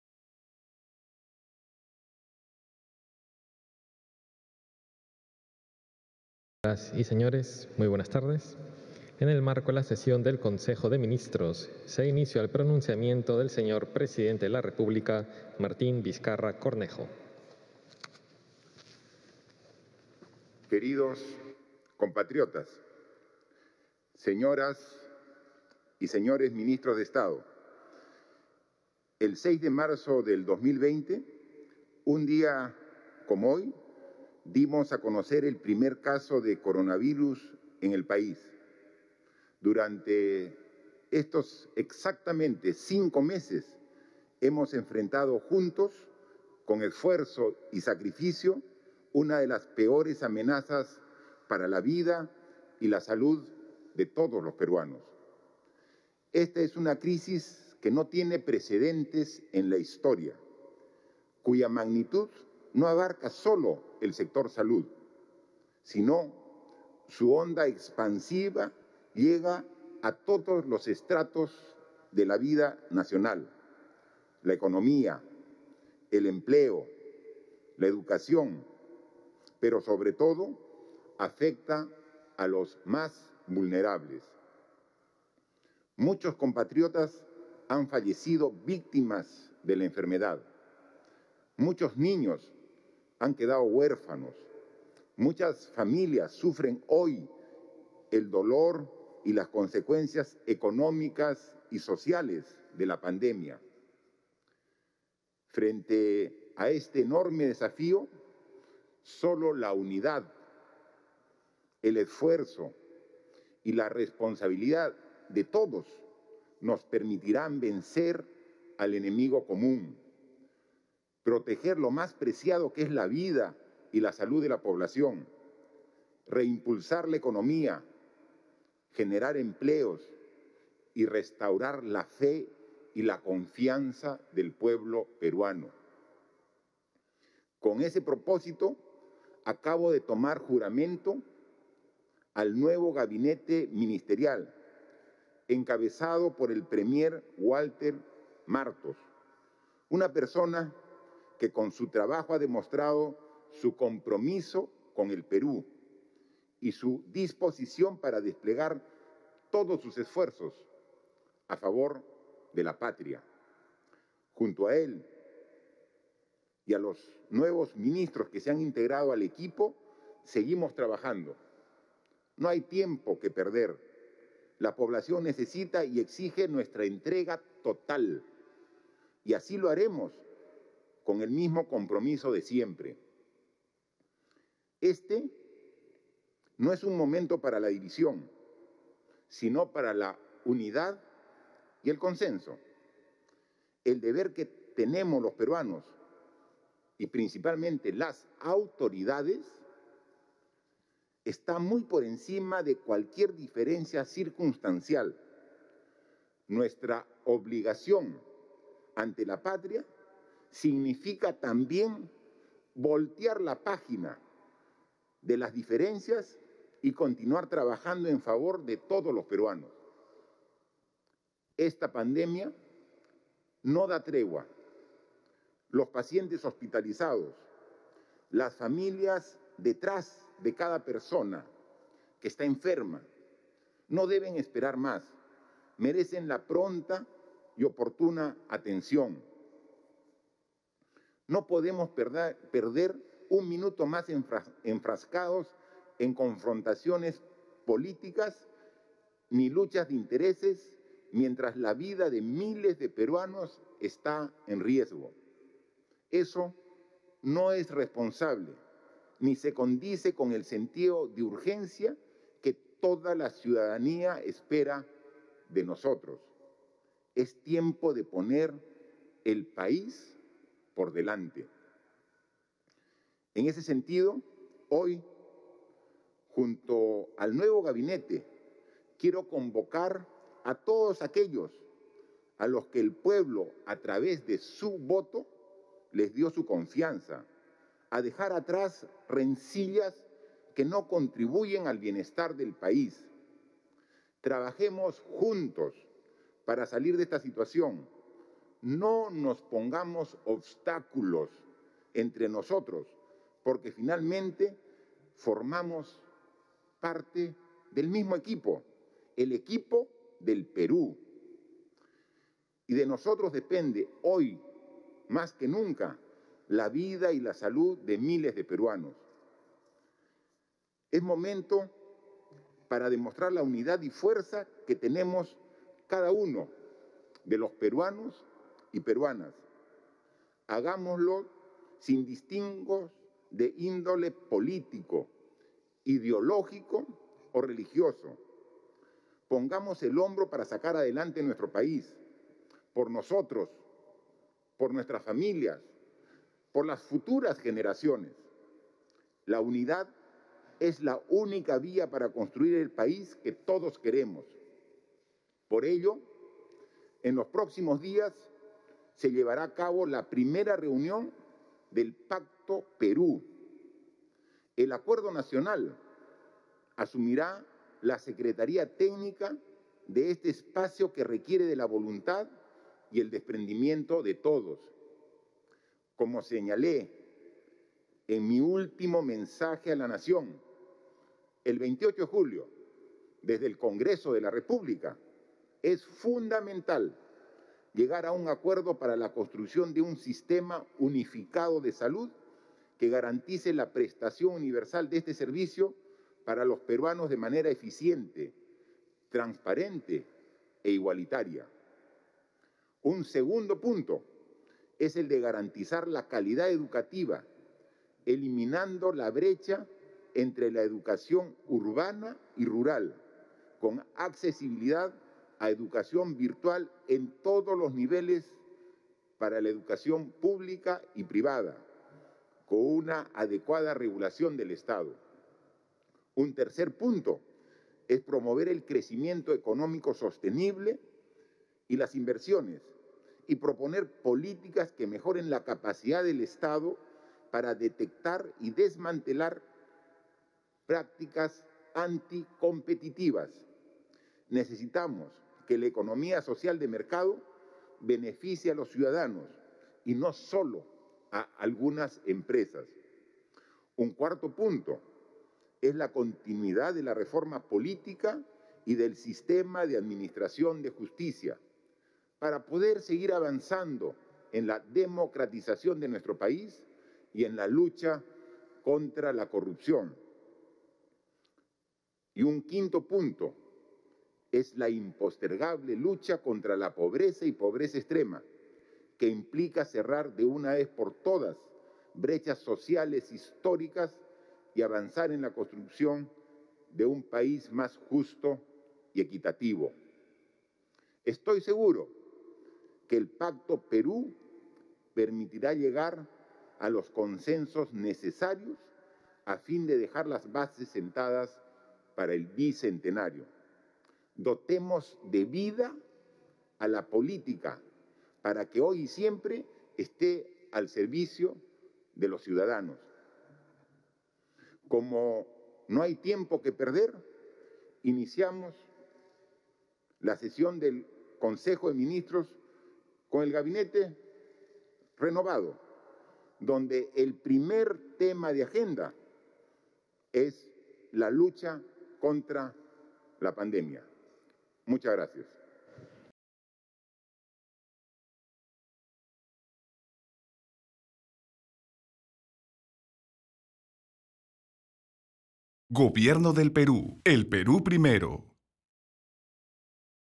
Presidente Martín Vizcarra brinda un pronunciamiento acompañado por su gabinete ministerial
Tras la juramentación del nuevo Gabinete Ministerial y en el marco del primer Consejo de Ministros, el mandatario invitó a todas las fuerzas políticas a unir esfuerzos y dejar de lado las desavenencias políticas e intereses para que, de manera articulada, reiniciar el trabajo en favor de la población y encaminar las acciones para combatir la pandemia.